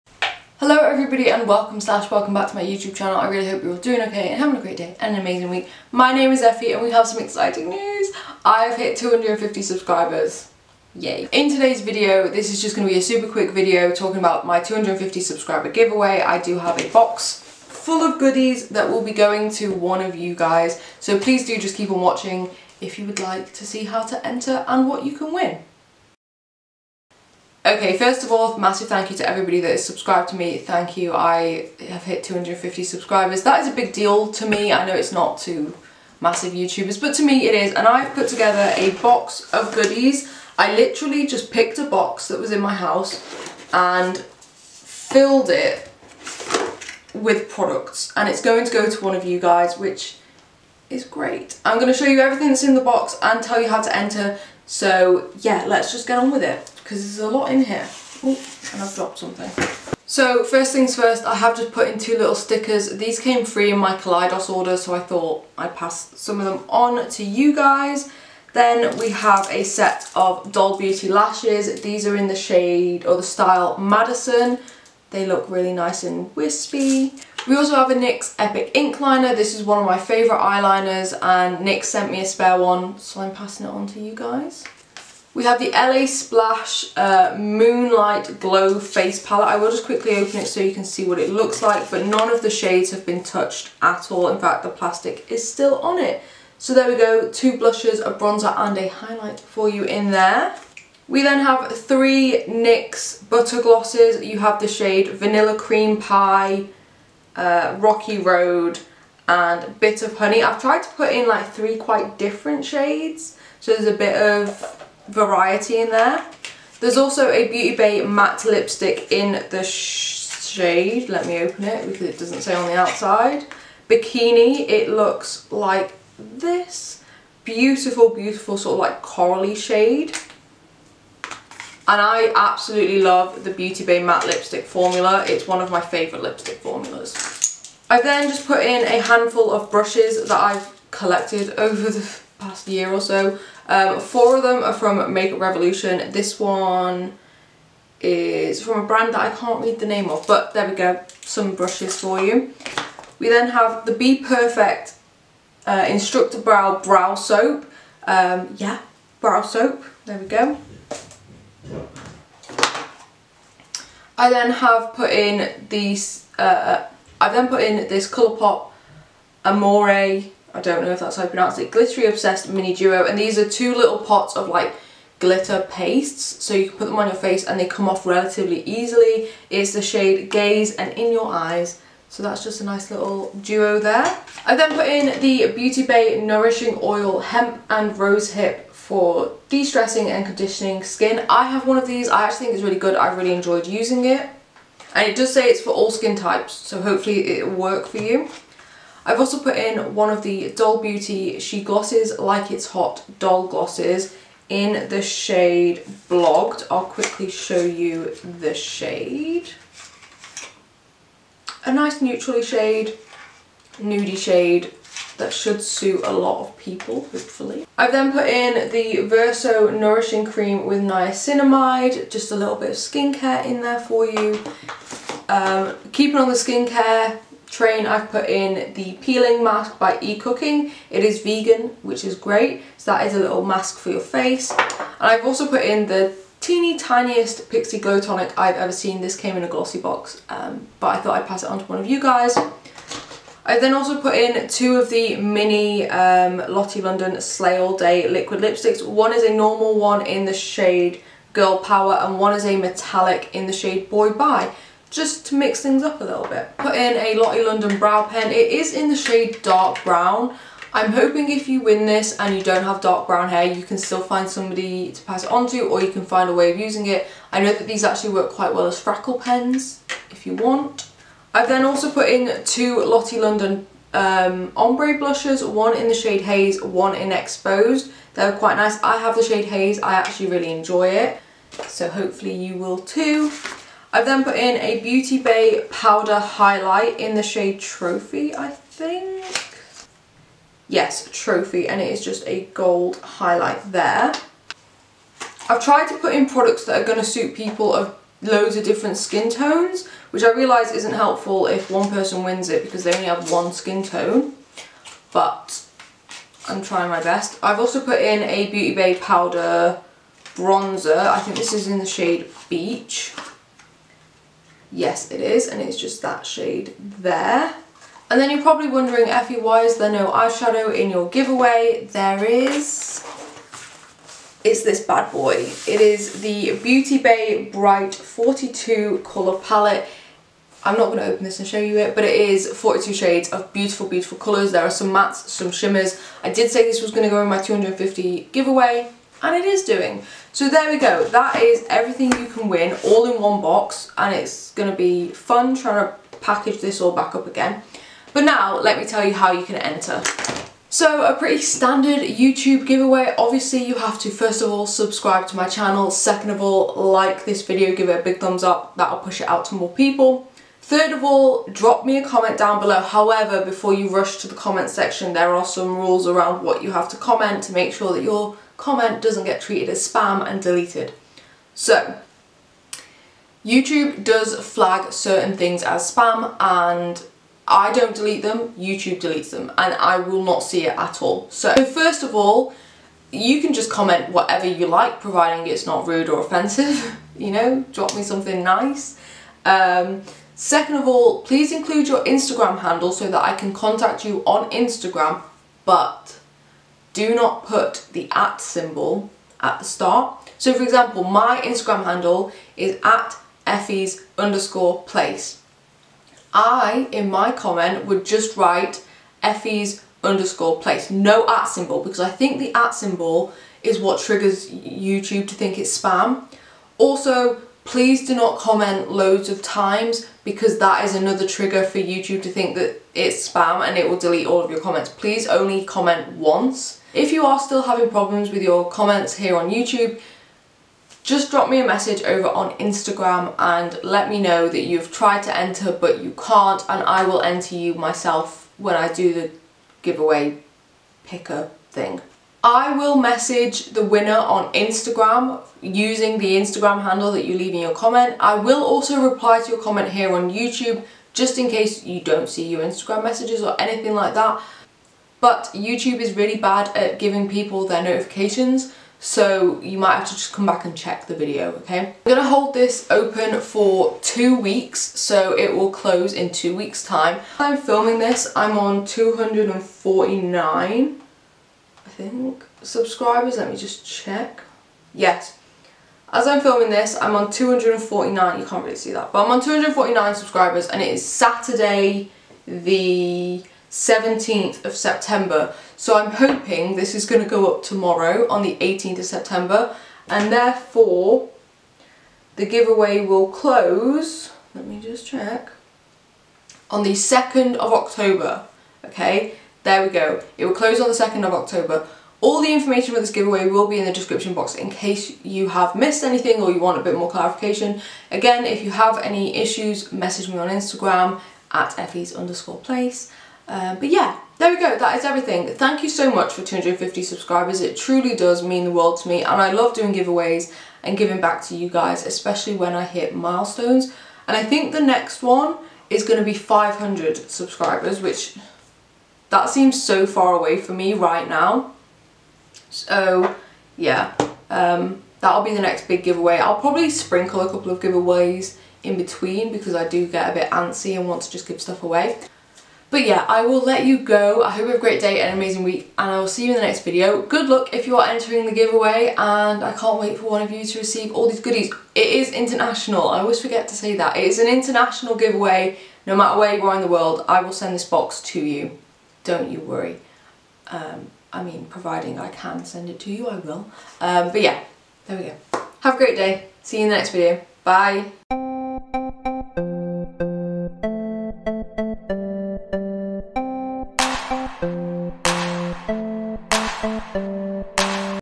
*CLOSED*250 SUBSCRIBER GIVEAWAY | INTERNATIONAL GIVEAWAY | MAKEUP AND SKINCARE | Effys Place
Hello Everybody! And today is a very happy video as we have reached 250 Subscribers! I know in the grand scheme of YouTube, 250 isn't a lot, but to me it means so much! I just want to say a little thank you in the form of a small giveaway!

How to enter:
3) Drop a comment with your Instagram handle (without the '@' symbol. E.g. 'Effys_place')
4) Don't comment more than once - it will be seen as spam by YouTube and deleted.

Rules:
2) Giveaway will run for 2 weeks unit midnight (UK time) on 2nd October 2022
3) This giveaway is in no way connected to any of the brands mentioned, YouTube or Instagram
4) All Items will be sanitised before sending
5) Once posted I am not liable for package.

I will contact the winner on Instagram and reply to their comment on Youtube once the giveaway has closed.